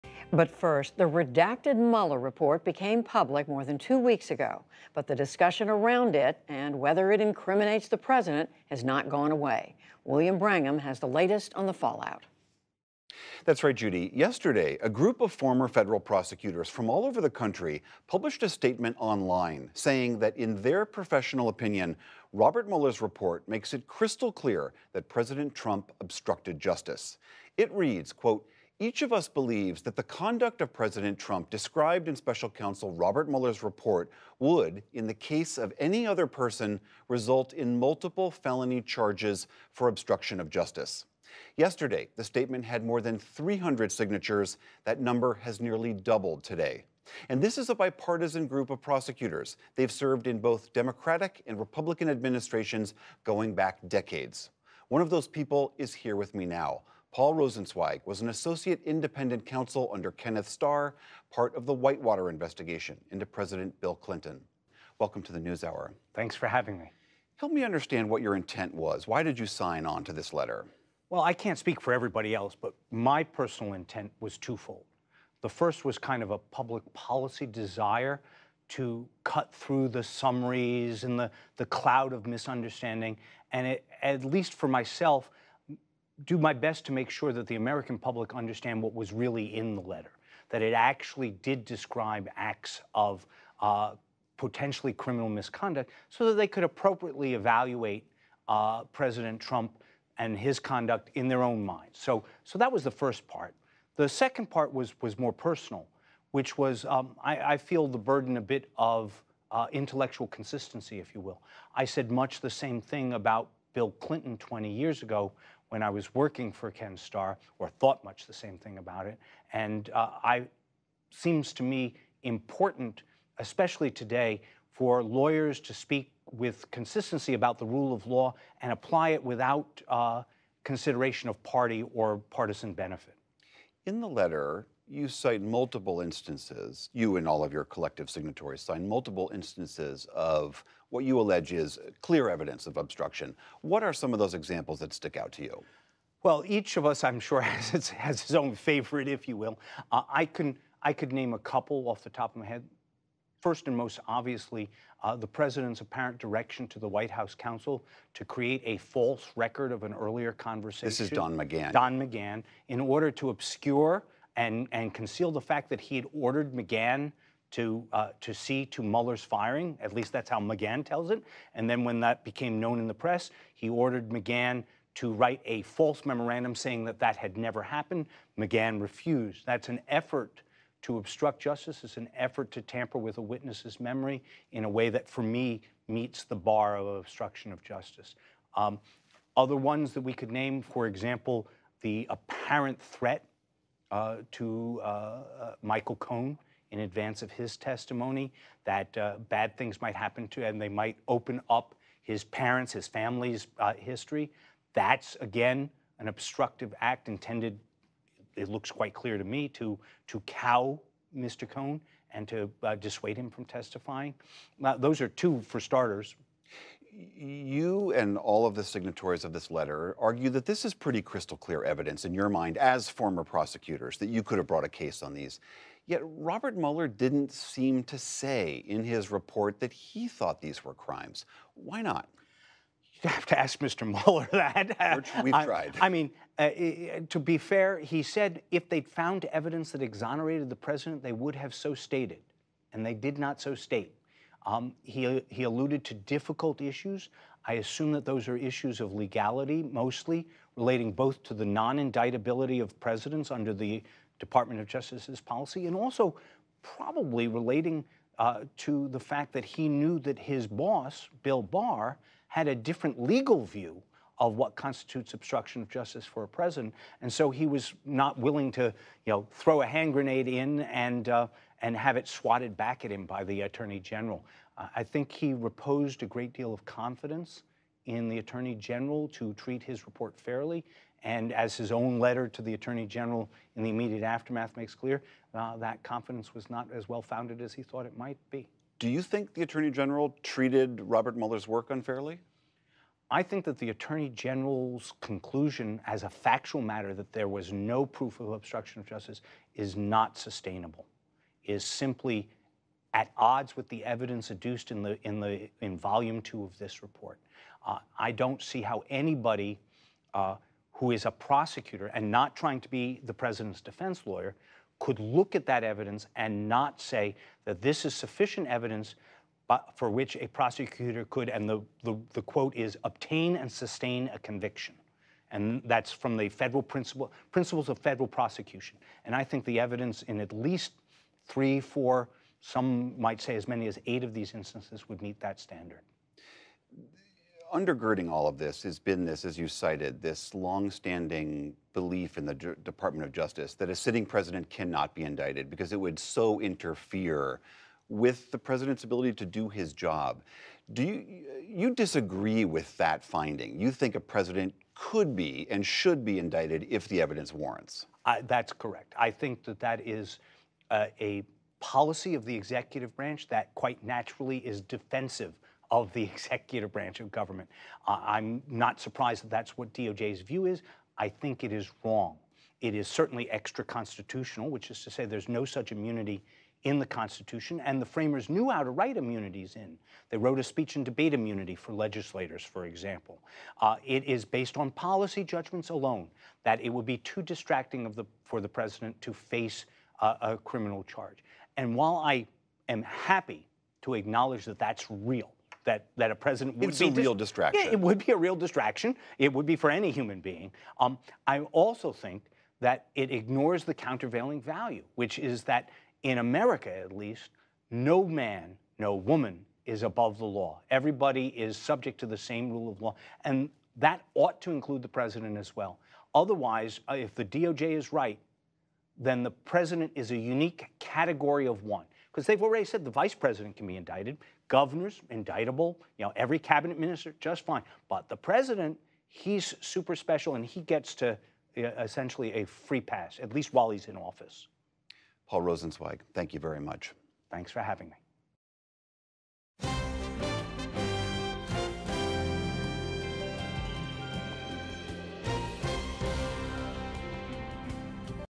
Why hundreds of former federal prosecutors disagree with Barr over Mueller report
The Mueller report continues to generate legal debate. Several hundred former federal prosecutors published a statement this week asserting that President Trump would have been charged with obstruction of justice, were he not the sitting president. William Brangham talks to Paul Rosenzweig, who worked with independent counsel Kenneth Starr on the Whitewater investigation of President Bill Clinton.